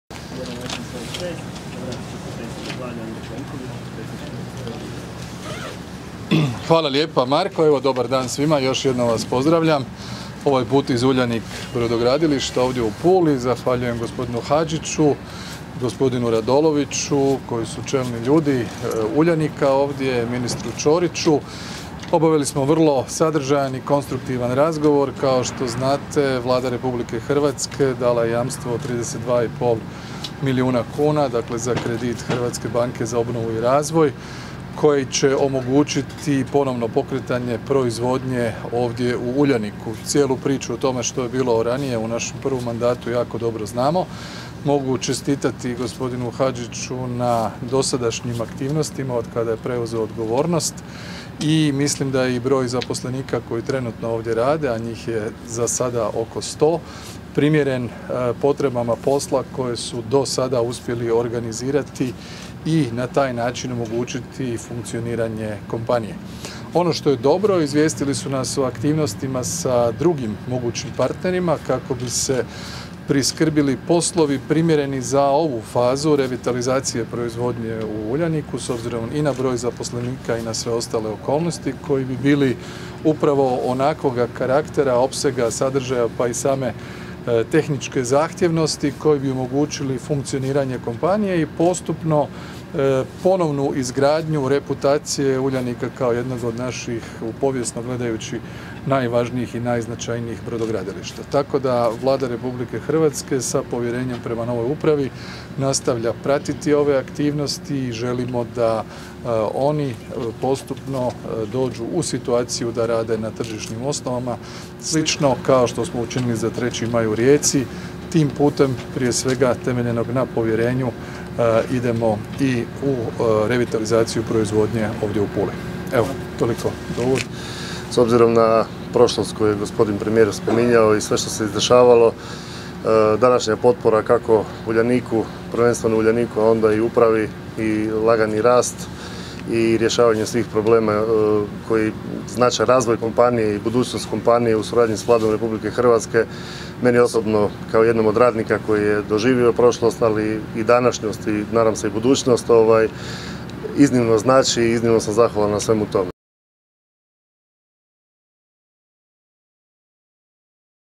Izjava premijera Plenkovića nakon posjete brodogradilištu Uljanik
Premijer Andrej Plenković u posjetu Istri gdje je potpisao nekoliko važnih projekata koji se odnose na bolnicu , Istarski Ipsilon , ribare ... Premijer se u Županiji sastao s Fabriziom Radinom, zamjenikom župana koji obnaša duznost župana i gradonaćelnikom Pule Borisom Miletićem. Posjetio je i brodogradilište Uljanik nakon čega je dao izjavu za medije.